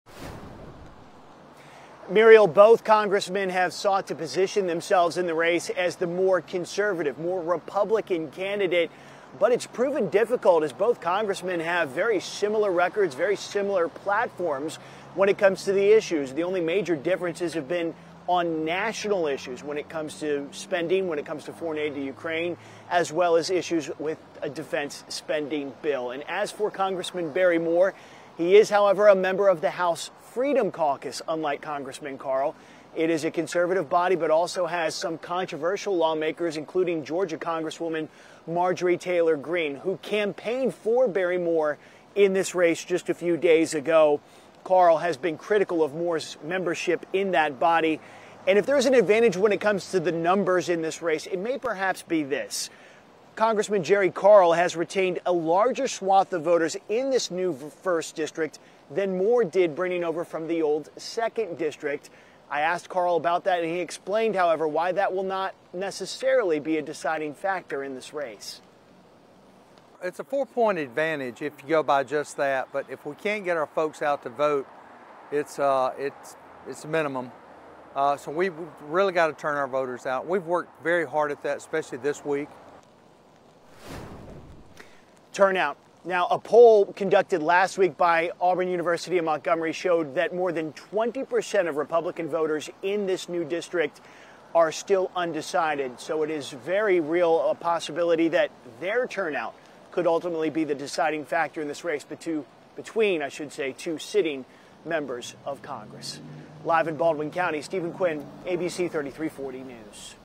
Alabama's Re-drawn District Pits GOP Incumbents Jerry Carl, Barry Moore in Super Tuesday Primary
Alabama's redrawn congressional districts has forced U.S. Rep. Barry Moore (R) to challenge Rep. Jerry Carl (R) in the state's 1st Congressional District